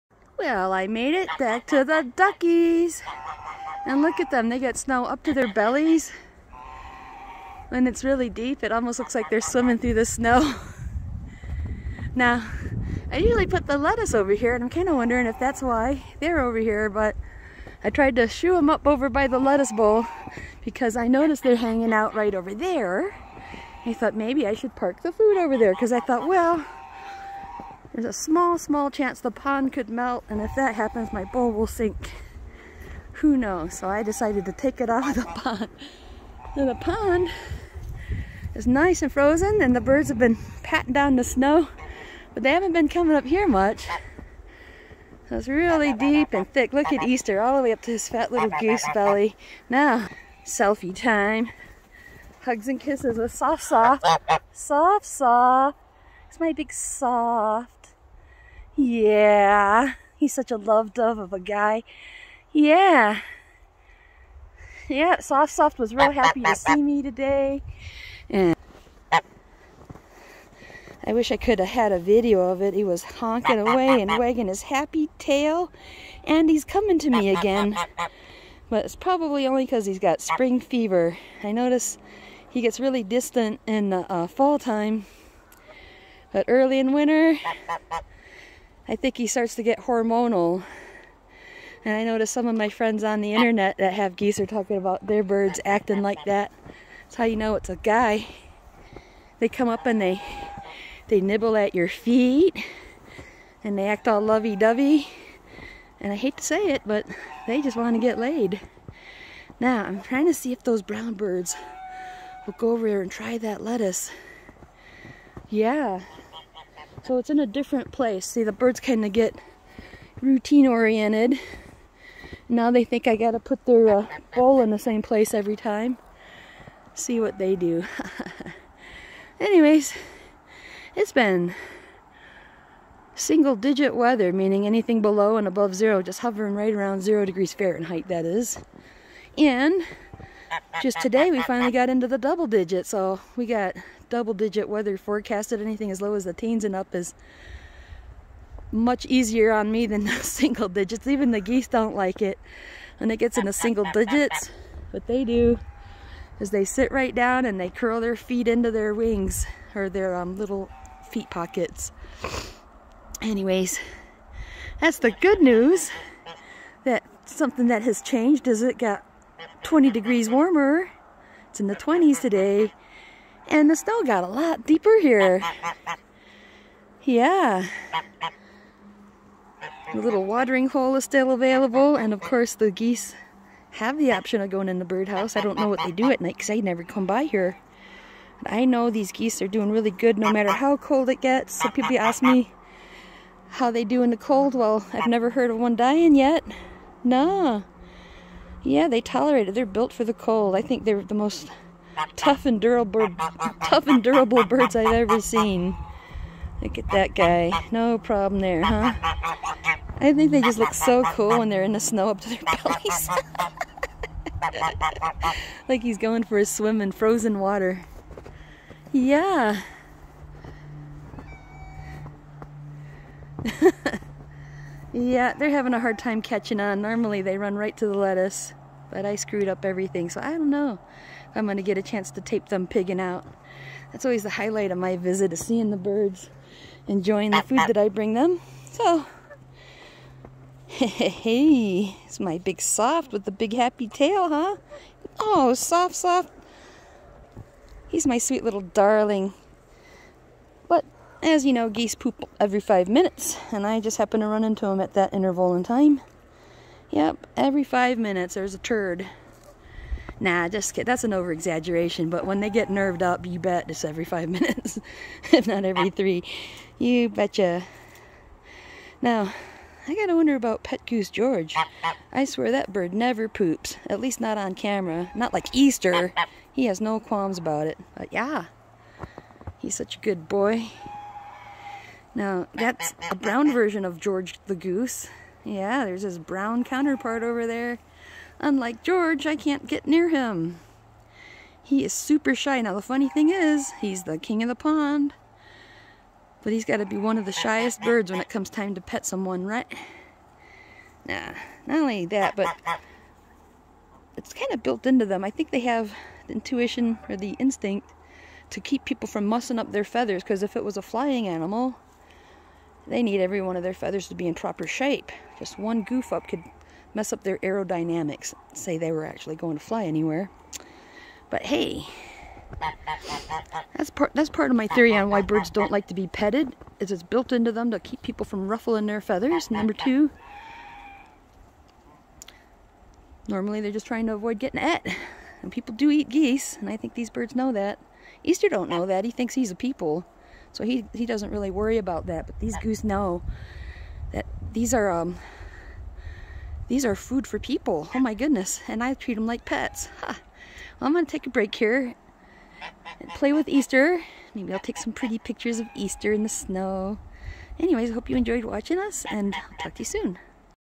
Keeping Food for Pets with Easter the Goose January 7 2022
My little joke, explaining a bird's natural inclination to keep a safe distance! Of coarse, I have no interest in eating any of them. However, bare in mind, people do hunt Canada geese and raise domestic breeds for food. These geese are at a rescue center where I come to visit every week. I raised Easter and have noticed sexual behavior early on, each year. It's the snowiest day we've had and super funny to see geese in snow up to their bellies lol!